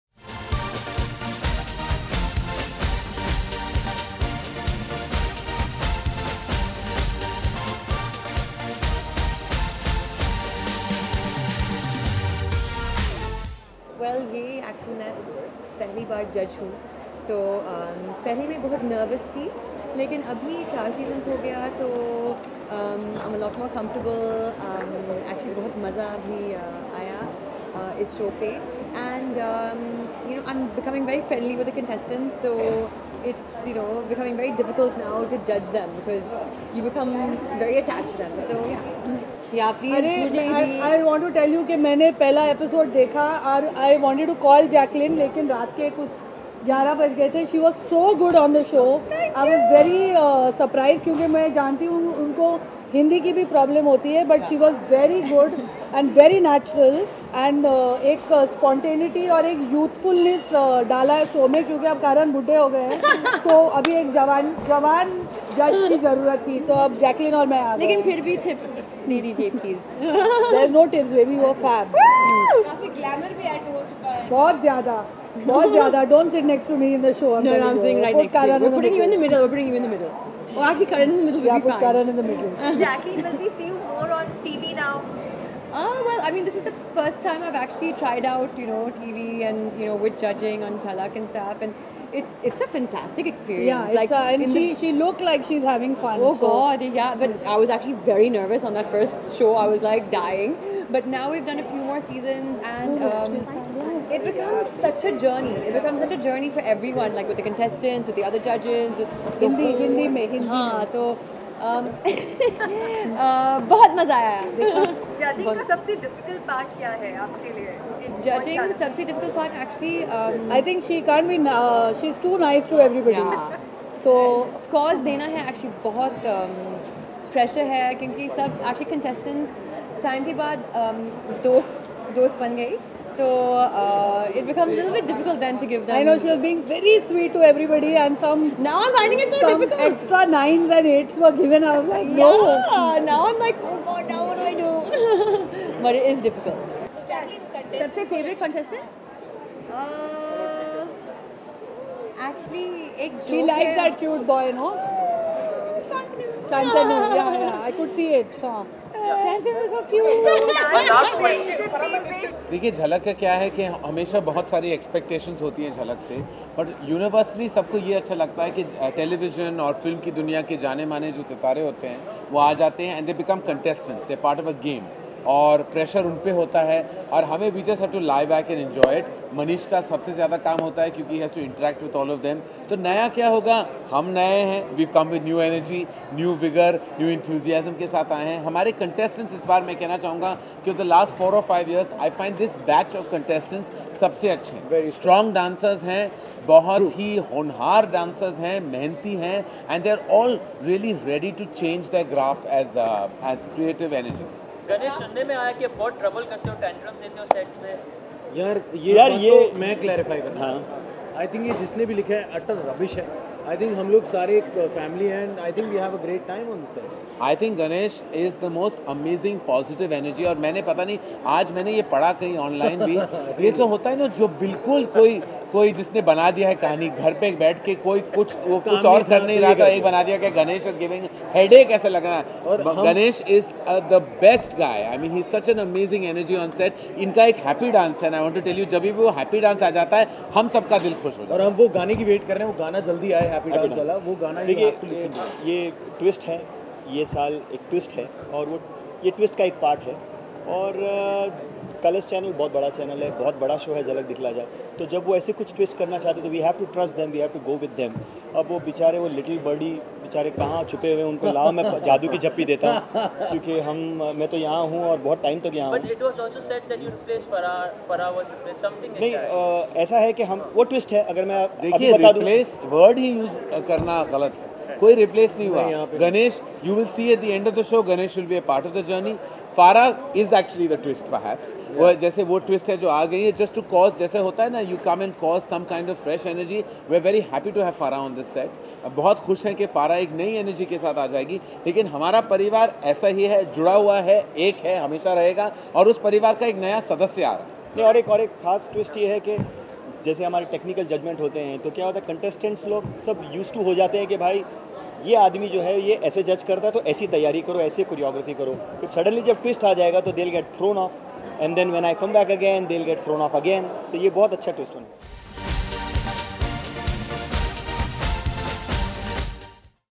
Jhalak Dikhhla Jaa Season 9 Media Interaction | Jacqueline Fernandez, Farah Khan, Karan Johar
Colorstv dance reality show Jhalak Dikhhla Jaa Season 9 Media Interaction with Jacqueline Fernandez, Farah Khan, Karan Johar, Ganesh Hegde and host Manish Paul.